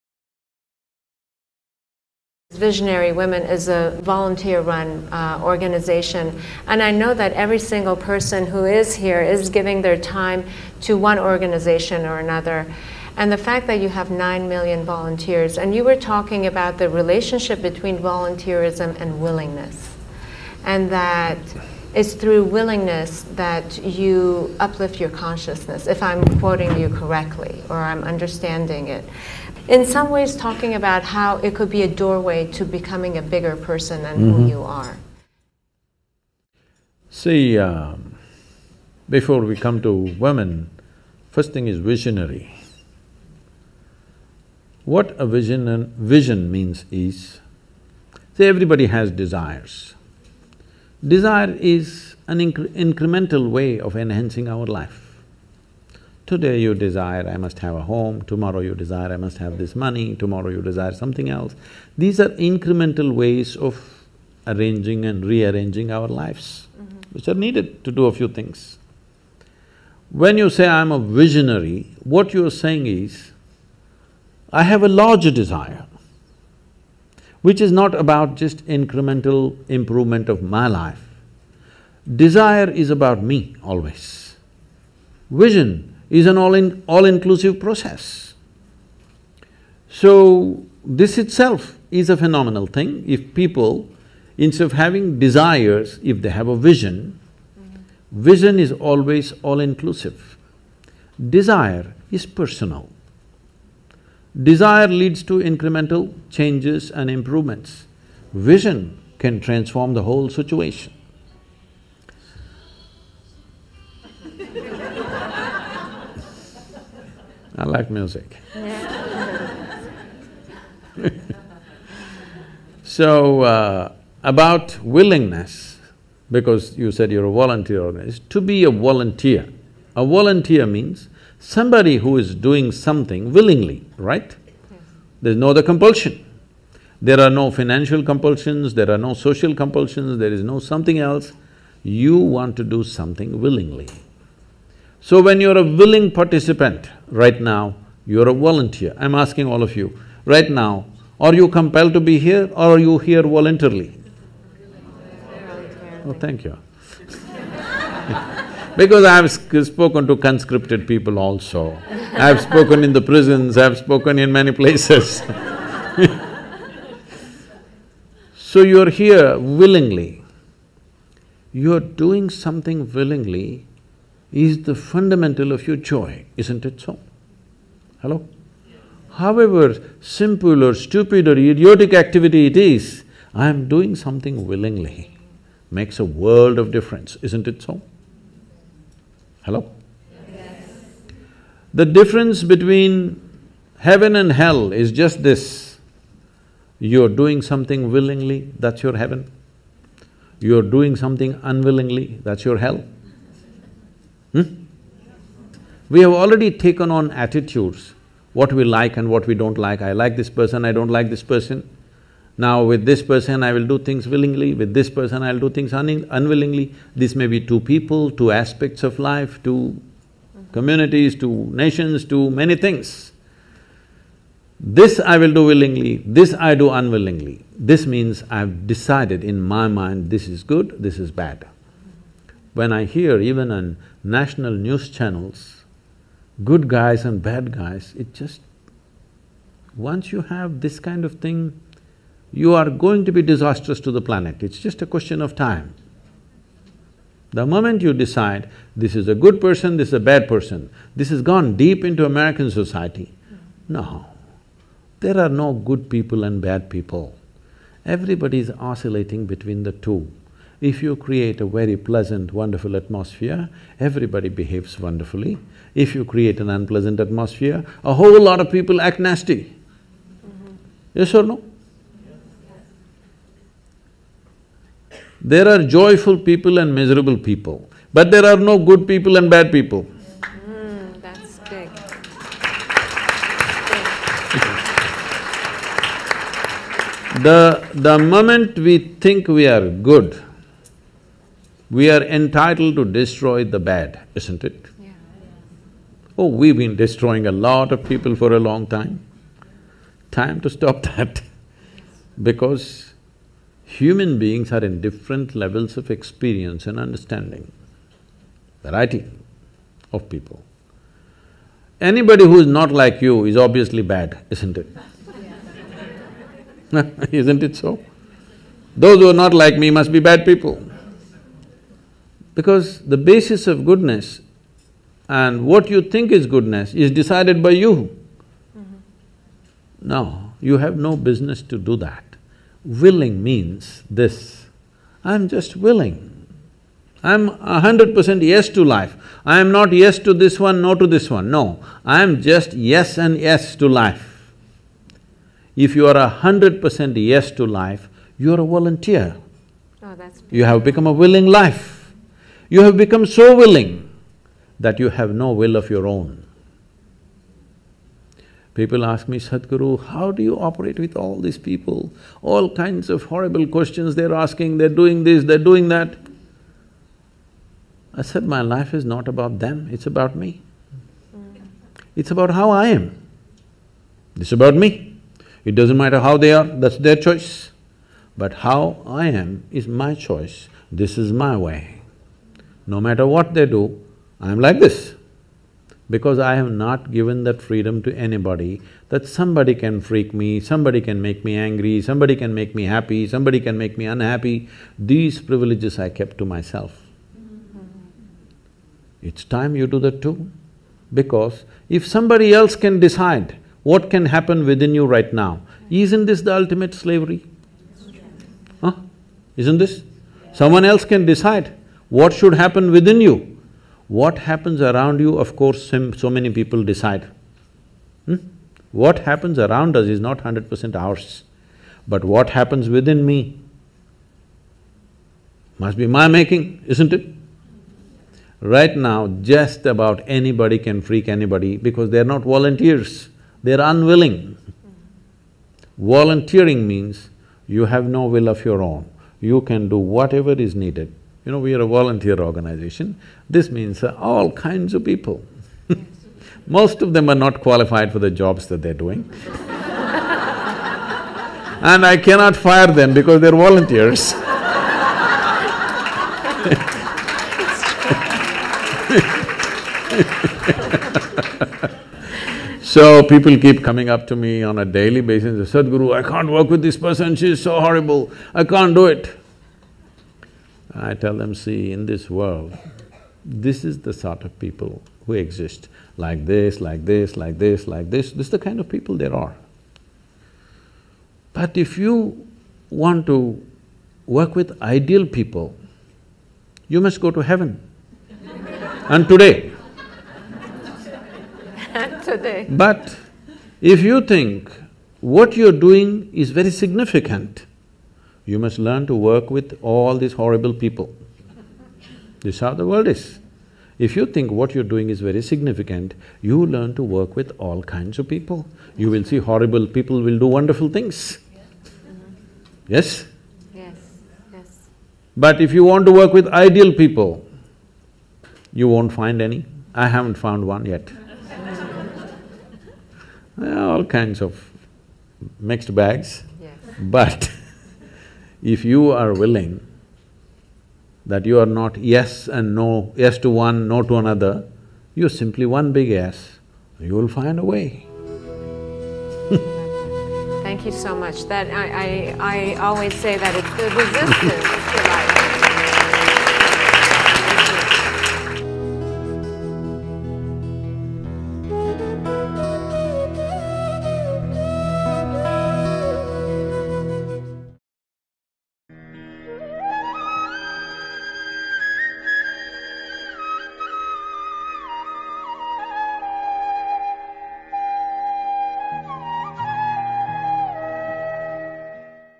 如何永不生气、不因别人心烦？| Sadhguru 萨古鲁
在与安吉拉 · 纳扎里安谈论，成为一个有远见的人和志愿者意味着什么的时候，萨古鲁探讨了主宰你生命的关键是如何变得有意愿——成为一个对生活纯粹有意愿的人！